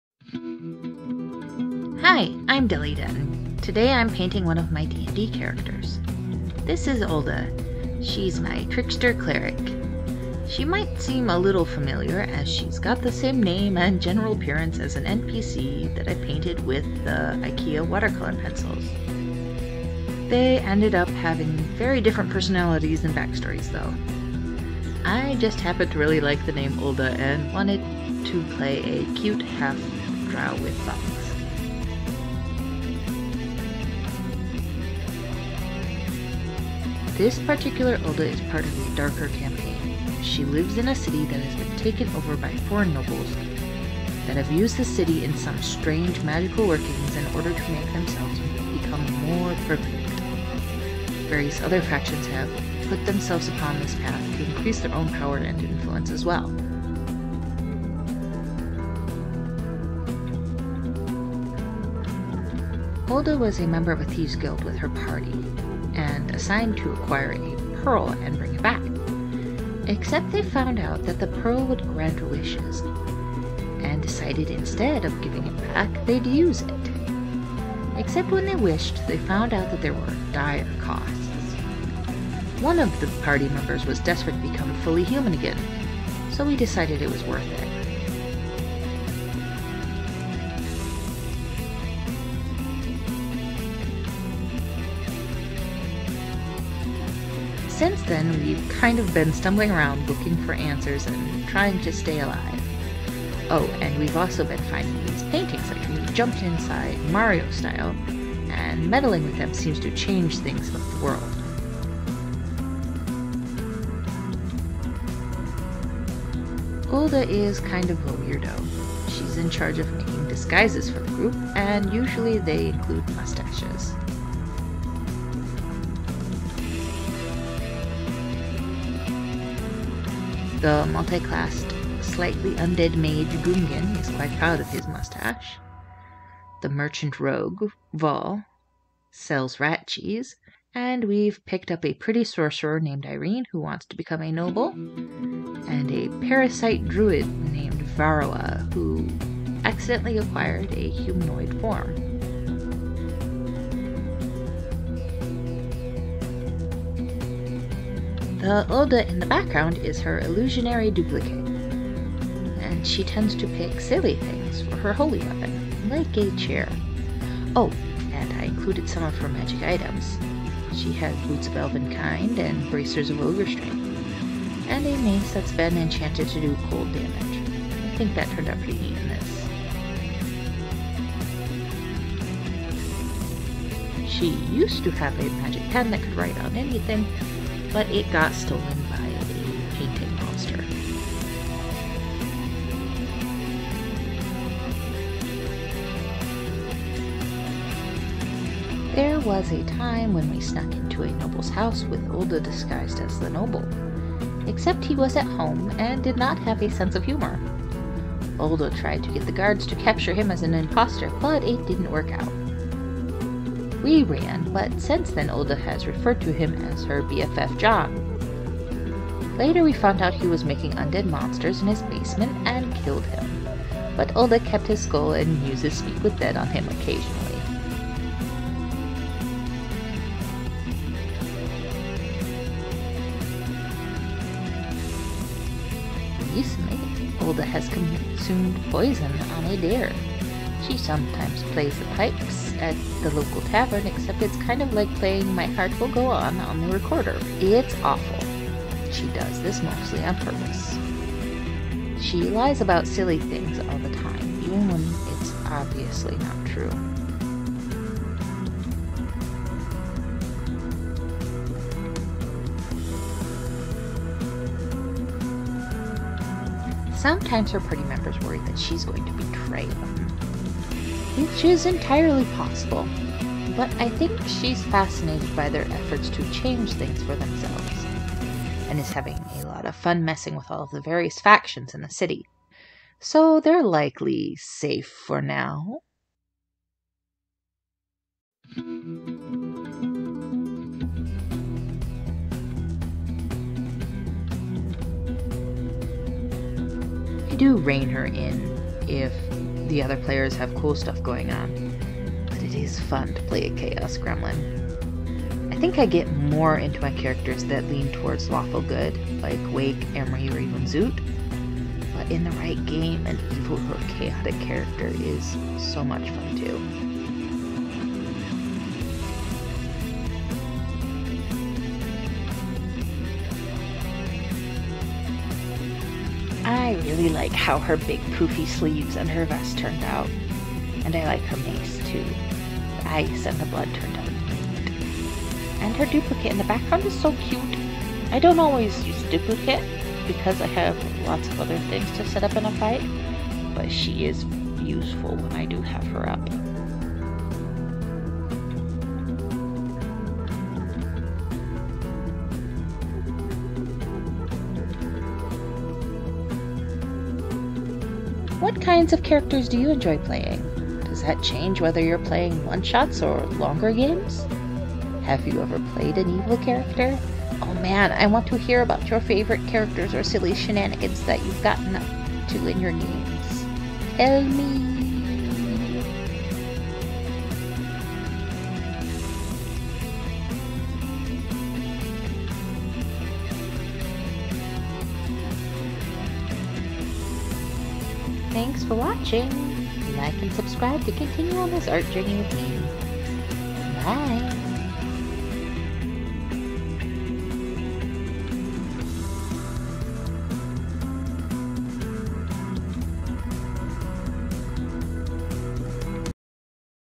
D&D Character Art: Trickster Cleric
Painting my Dungeons and Dragons character, the trickster Cleric Ulda!

~Materials~
Kurotake Water Colors
Prismacolor Pencils
Posca Paint Markers